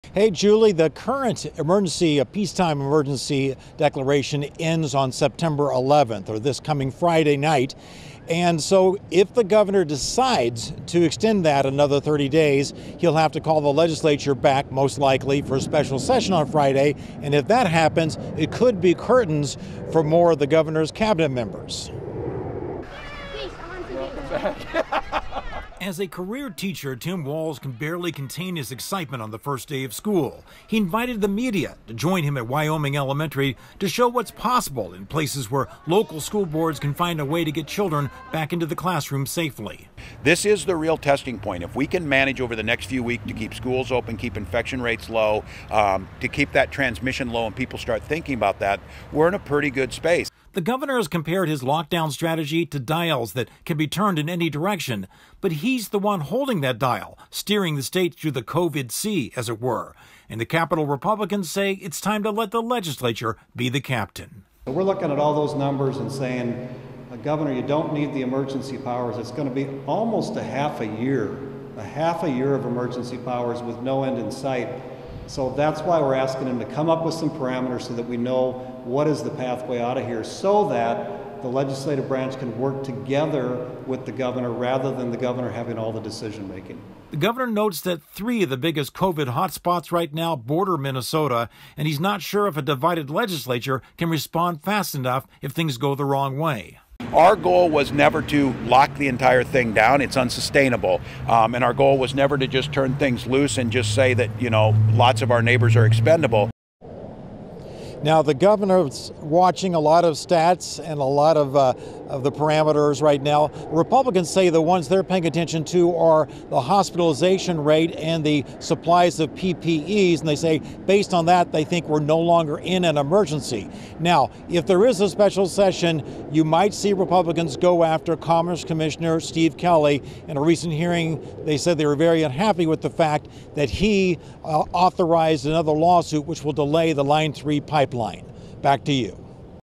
Walz sees school launch as key test for Minnesota
Gov. Tim Walz views the first two weeks of school as a key test of Minnesota's COVID control strategy, but he's also being pressed to give up his emergency powers